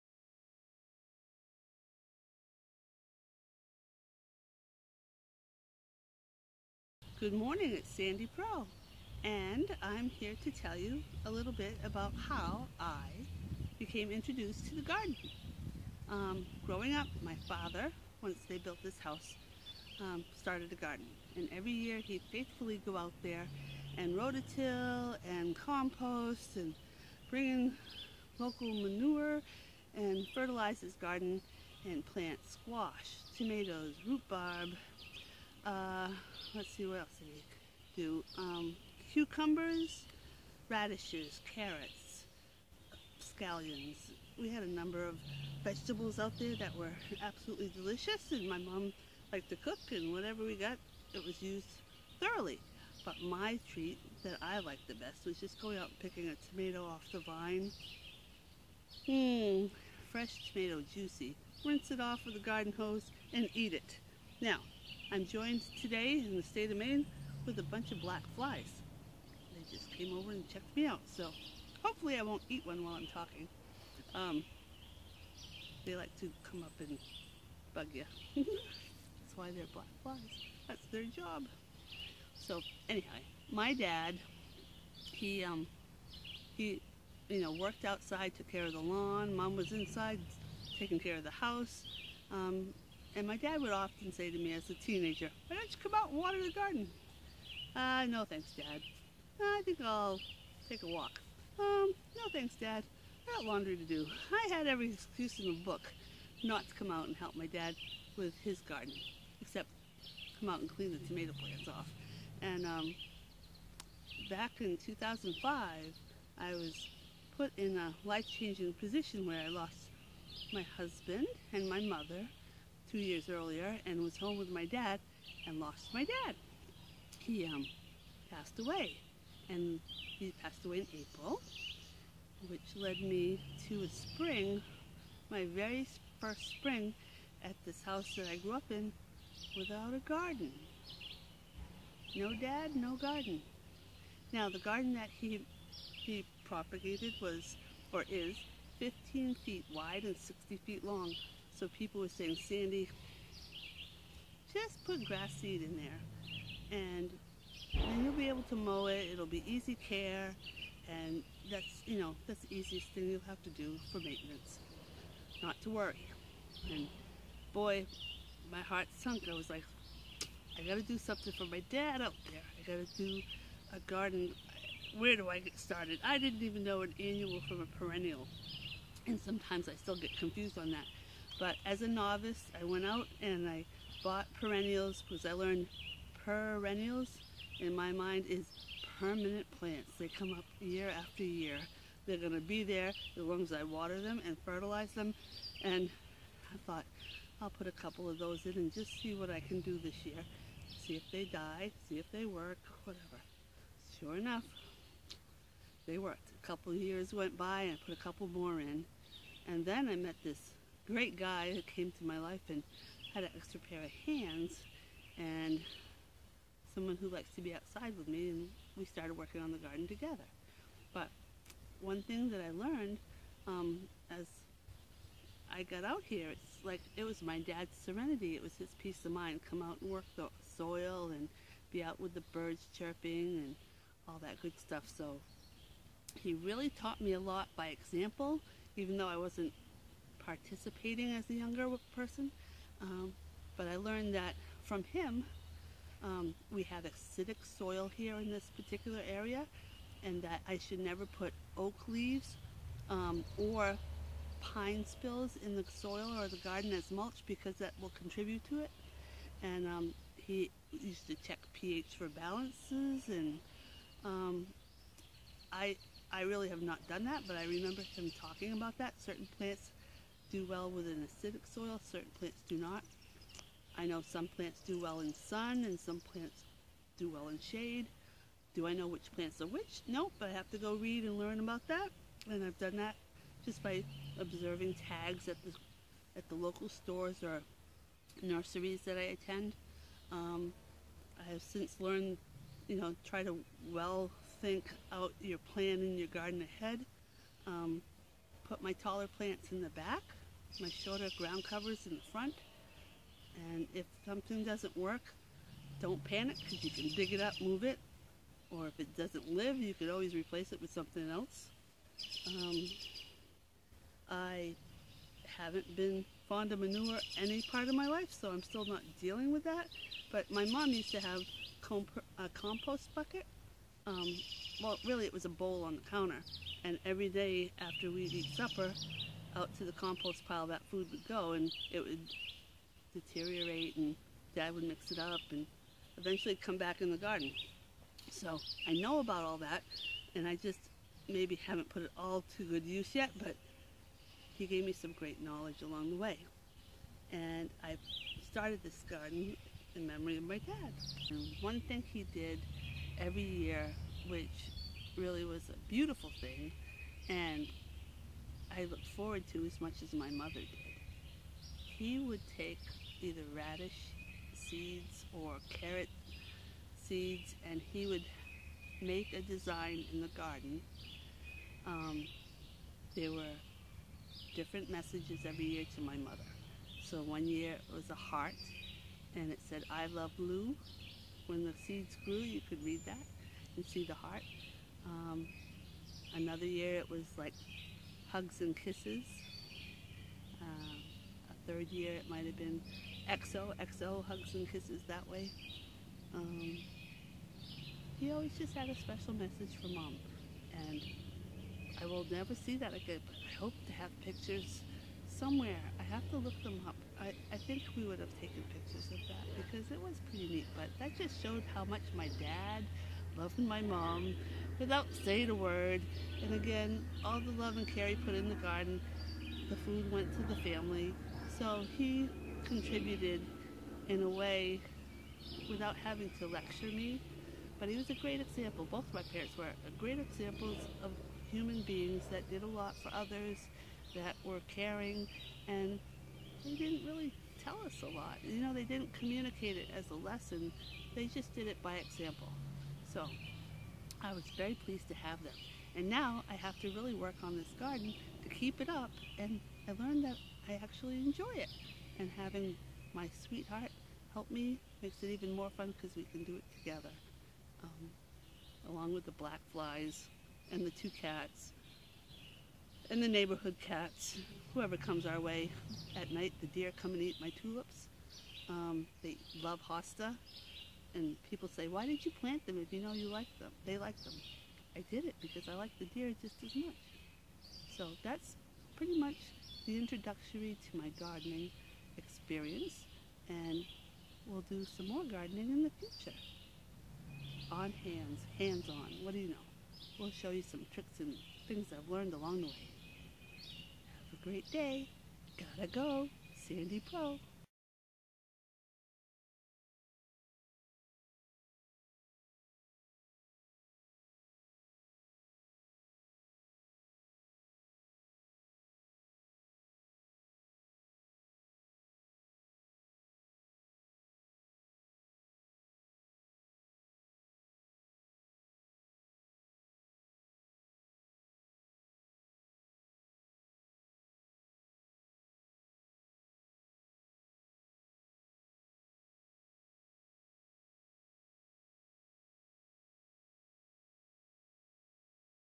How Sandy became one who gardens. 05-23-16
At the age of 43, I was faced with a life changing question. Grass or Garden ?  This story will tell you how I became one who gardens. :)